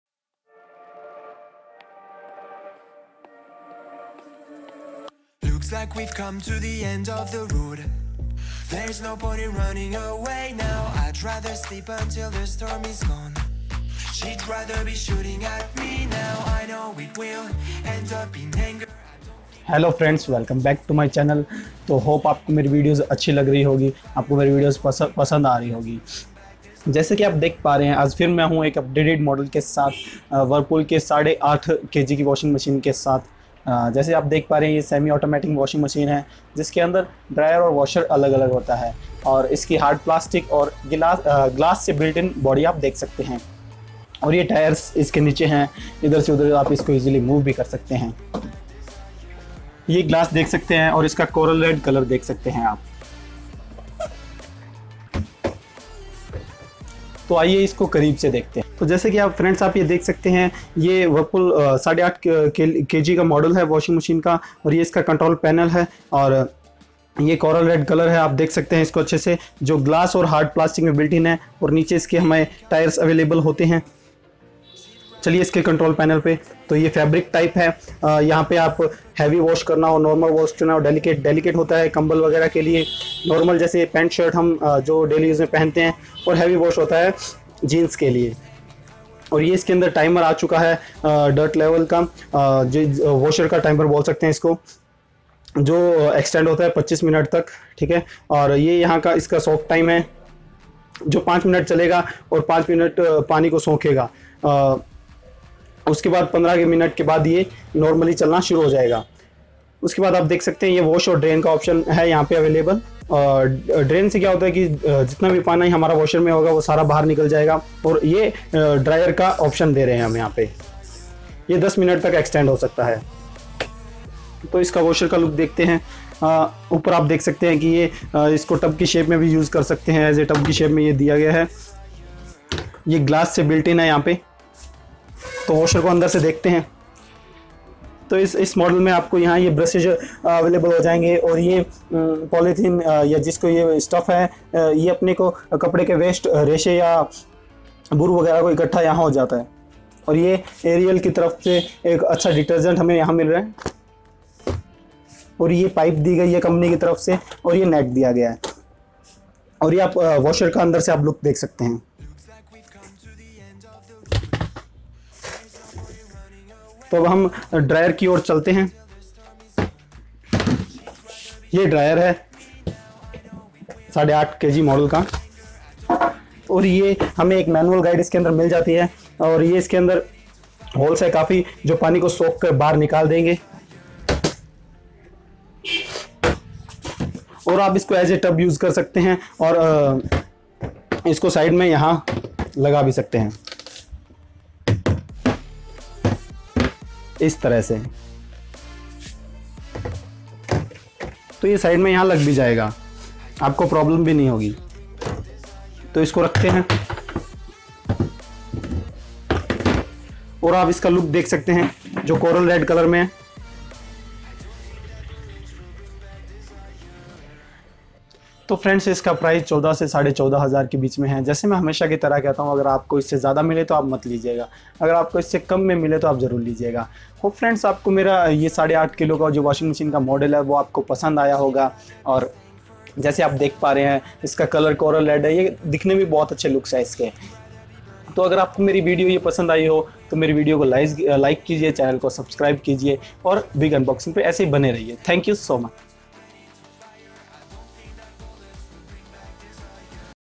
Whirlpool 8.5KG Washing machine || whirlpool semi automatic washing machine || UNBOXANDREVIEWINHINDI
So here i am with my series of whirlpool washing machine review and unboxing in hindi so this is my 4th video hope this video is helpful for you friends watch it enjoy it like it and subscribe my channel and for any enquiry and question you can put it in comments or mail me.